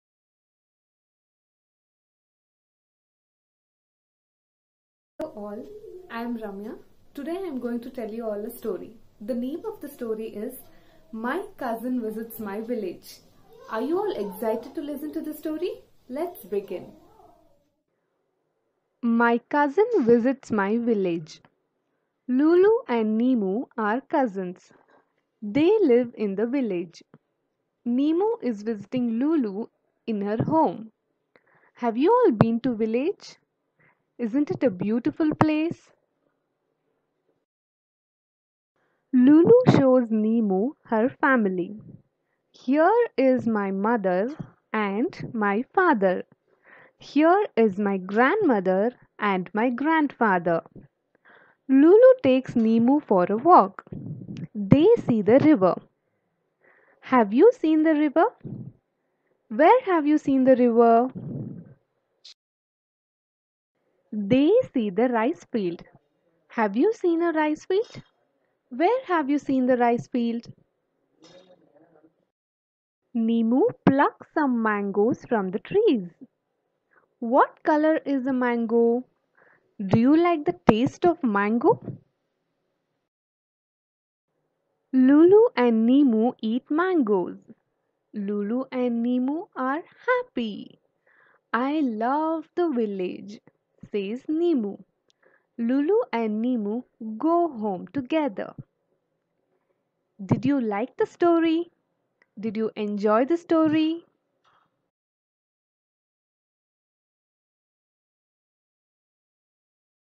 Story Time | My cousin visits my village
Watch the video to see how to narrate 'My cousin visits my village' story.